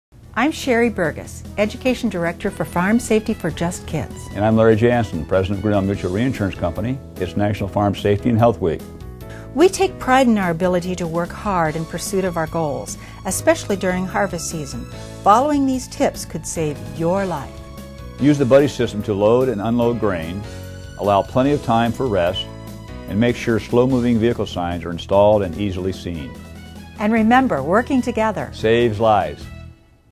National Farm Safety & Health Week: Working Together for Safety in Agriculture
Working Together for Safety in Agriculture is the focus of National Farm Safety & Health Week is September 15-21, 2013. During harvest season, try to get plenty of rest despite the long hours. Use the buddy system to load and unload grain and make sure slow-moving vehicle signs are installed and easily seen.

Remember, working together saves lives.